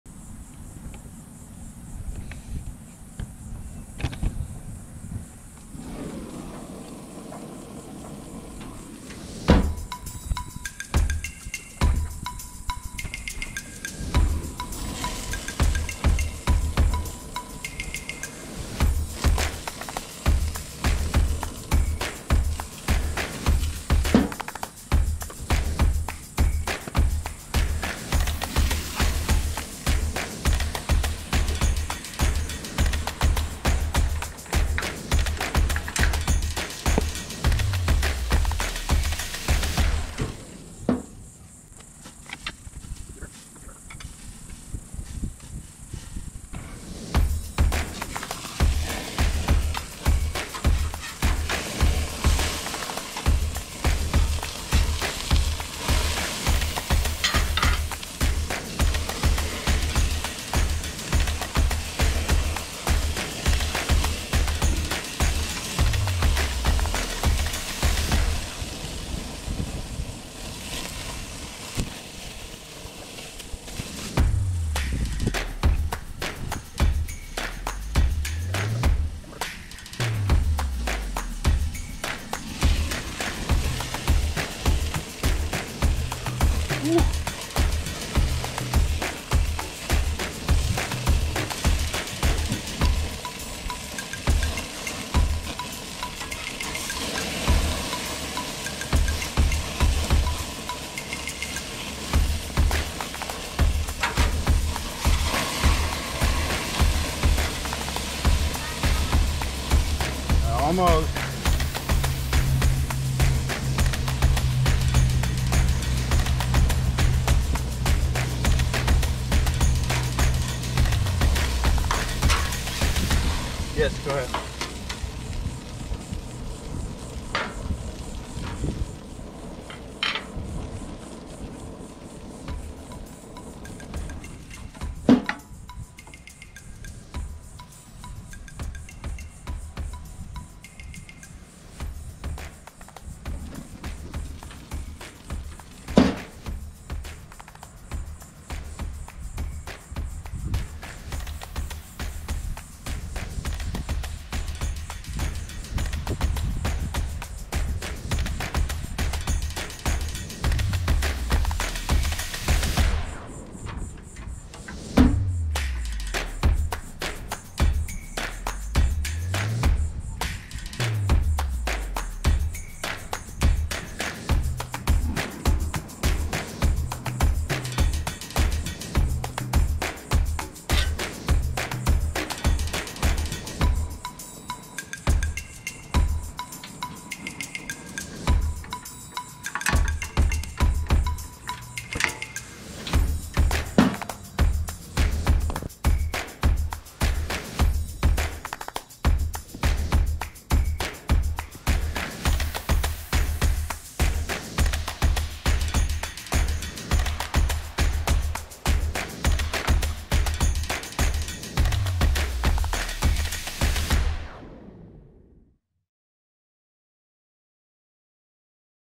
The 5 minute cheeseburger on the cheapest gas grill I could find.
This is a short and simple video of me making a cheeseburger on the cheapest gas grill I found at Wal-Mart in 2022. The grill cost $107 - of course, you have to get the gas cylinder and gas too, but $107 for a three-burner gas grill? This is the second cook on the 5th of July. I made steaks on the 4th and actually over-cooked them on this little bad boy.

Warning: Assembly of aforementioned grill is a challenge. 🔥🔥